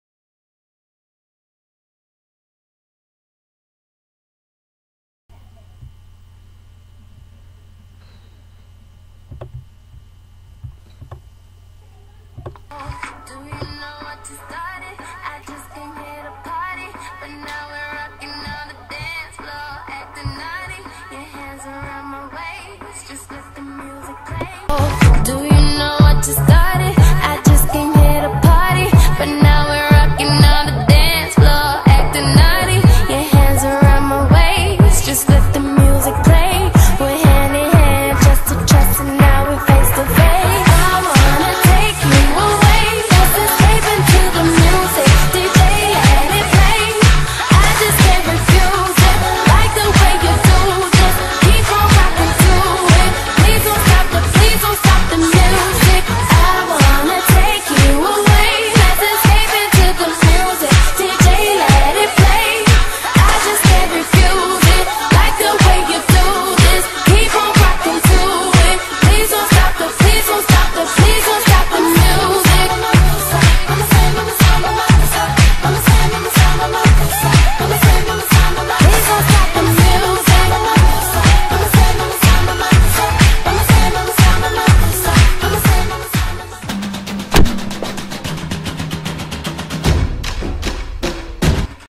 1 Minute of Fame
ALL SONGS USED SOLELY FOR ENTERTAINMENT...
had to make a minute dance solo for end of school celebration, and unfortunately was only limited to one minute of time!!!
enjoy it. hopefully